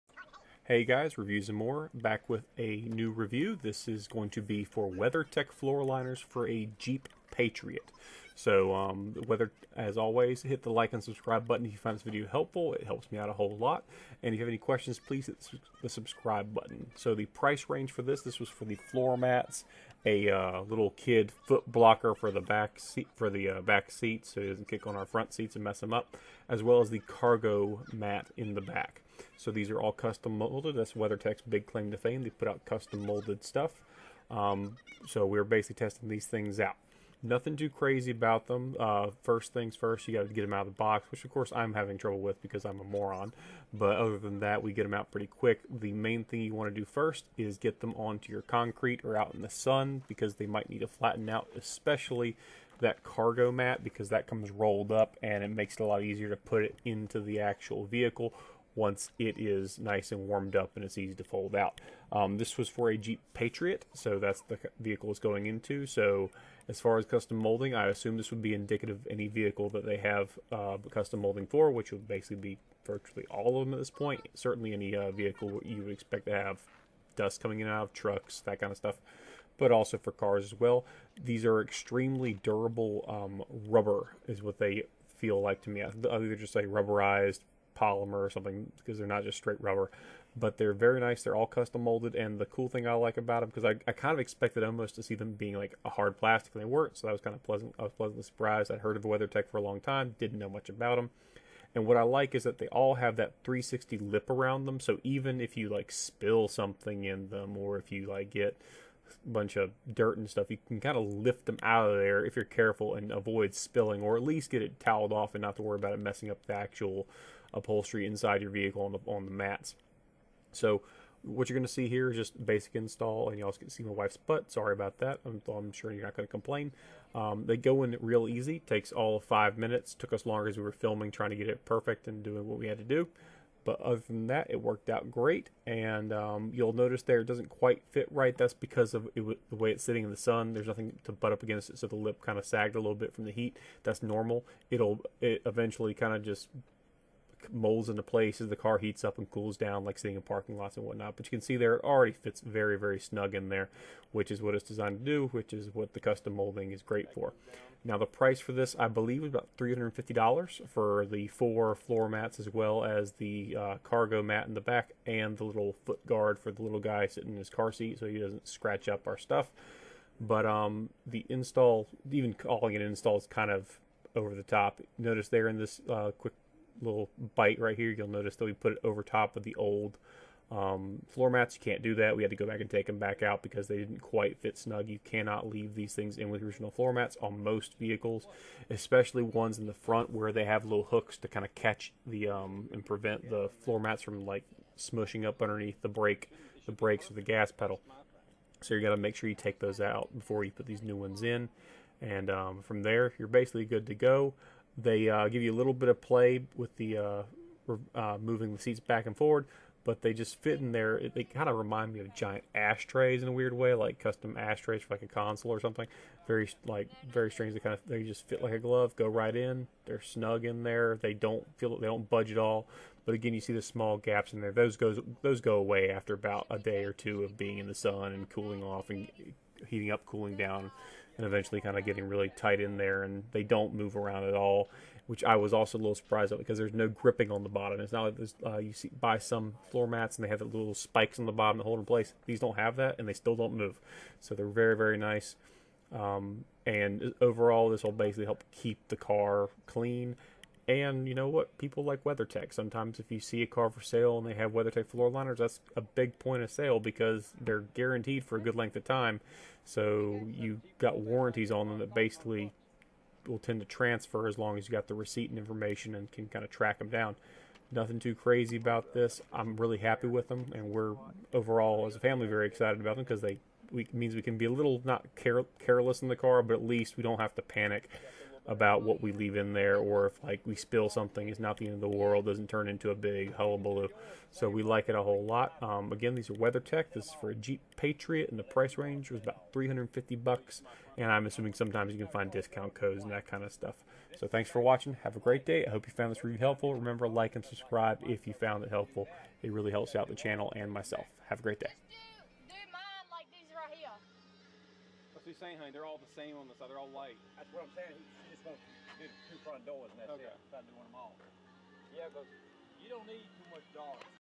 WeatherTech FloorLiner mats review for Jeep Patriot
all in price on floor mats, cargo mat, and seat protector (kids kicking the back seat product ($350ish)

Really cool items, worth every penny and fit great. Give them a day or two to settle into shape once you have them installed, and dont leave in the old floor mats.

Great product